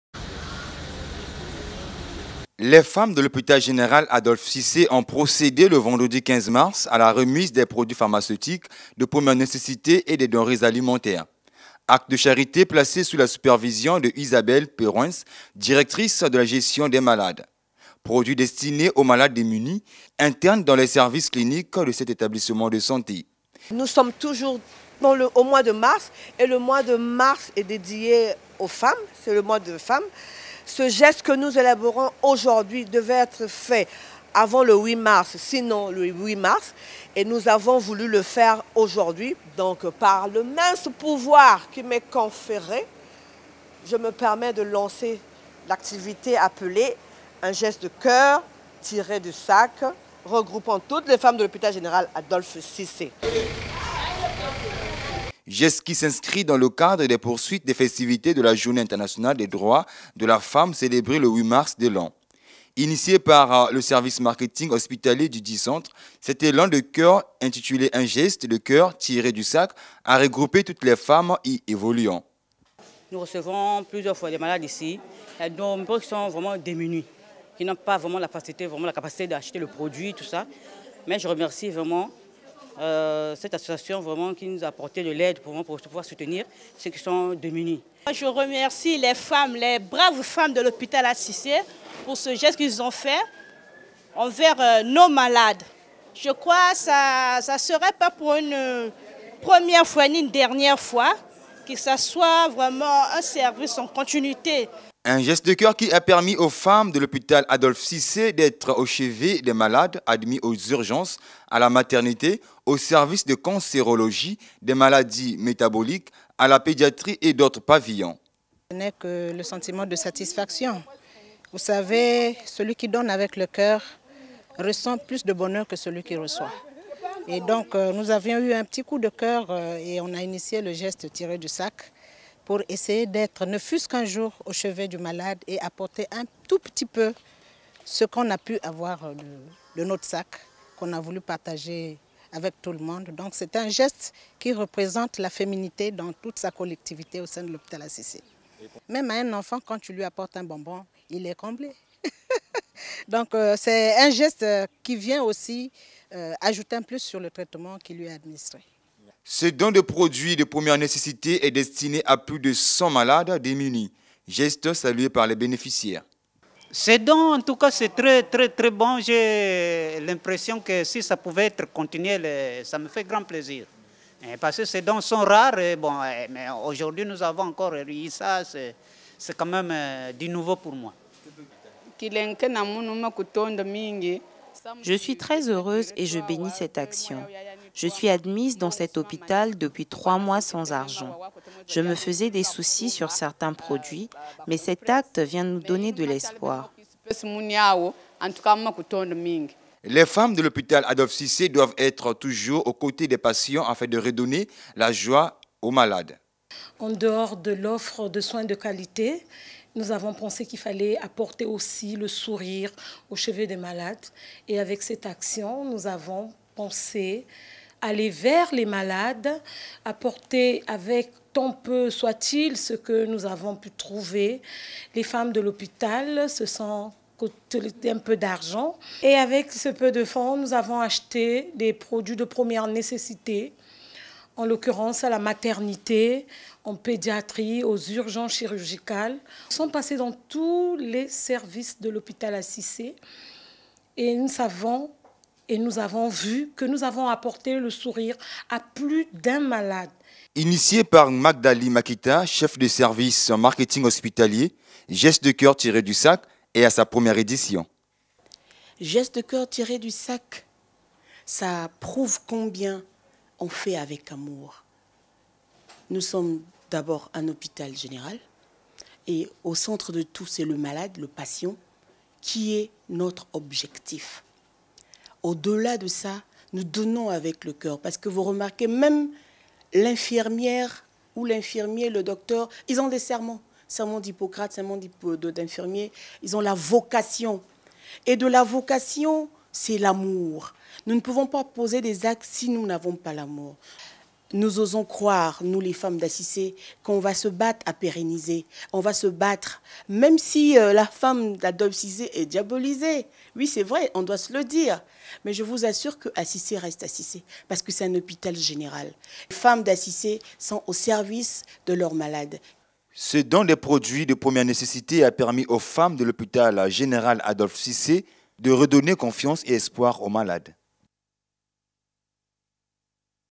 Don des produits pharmaceutiques aux malades à Pointe Noire
Les femmes de l’hôpital Général Adolphe Sicé ont procédé le vendredi 15 mars à la remise des produits pharmaceutiques de première nécessité et des denrées alimentaires. Acte de charité placée sous la supervision de Isabelle PURUEHNCE, directrice de la gestion des malades. Produits destinés aux malades démunis, internes dans les services cliniques de cet établissement de santé.
Geste qui s’inscrit dans le cadre des poursuites des festivités de la journée Internationale des droits de la femme célébrée le 08 mars de l’an. Initié par le service Marketing hospitalier dudit centre, cet élan de cœur intitulé Un Geste de Cœur tiré du Sac a regroupé toutes les femmes y évoluant. Ce Don est destiné à plus de 100 malades démunis.